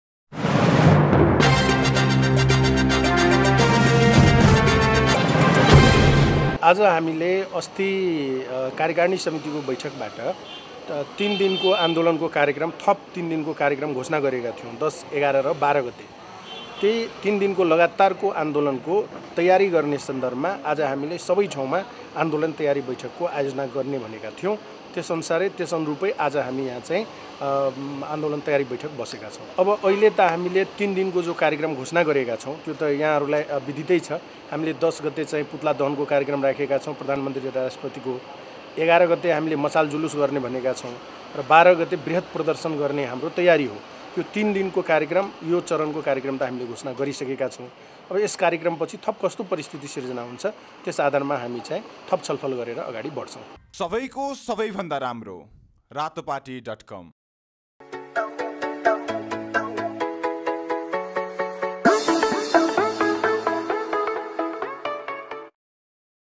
जसपा नेपालले संसद विघटनको विरुद्धमा प्रधानमन्त्री र राष्ट्रपतिको पुत्ला दहन गर्ने
जसपा नेपाल
जनता समाजवादी पार्टी, नेपाल